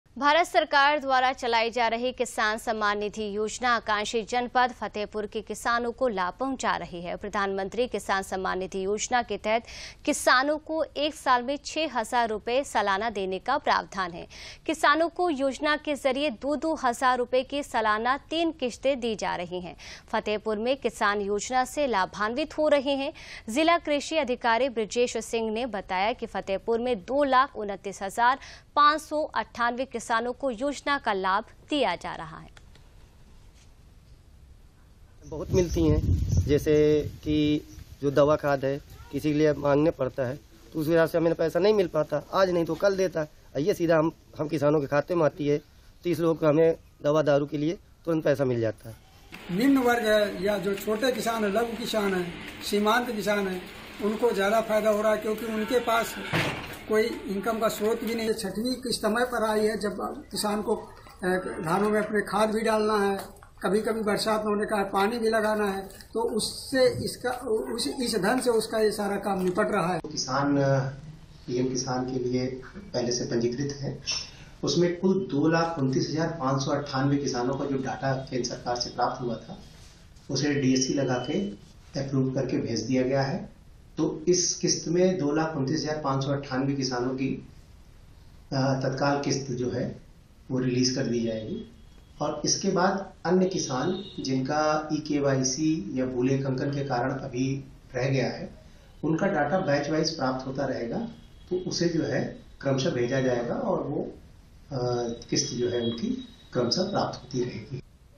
Fatehpur में किसान सम्मान निधि योजना से किसानों को मिल रहा है लाभ
भारत सरकार द्वारा चलाई जा रही किसान सम्मान निधि योजना आकांक्षी जनपद फतेहपुर के किसानों को लाभ पहुंचा  रही है। प्रधानमंत्री किसान सम्मान निधि योजना के तहत किसानों को 1 साल में 6000 रुपए सालाना देने का प्रावधान है। किसानों को योजना के जरिये दो -दो हजार रुपये की सालाना 3किस्तें दी जा रही है।  फतेहपुर में किसान योजना से लाभान्वित हो रहे है जिला कृषि अधिकारी बृजेश सिंह ने बताया कि फतेहपुर में 02लाख 29 हजार 5 सौ 98 किसानों को योजना का लाभ दिया जा रहा है।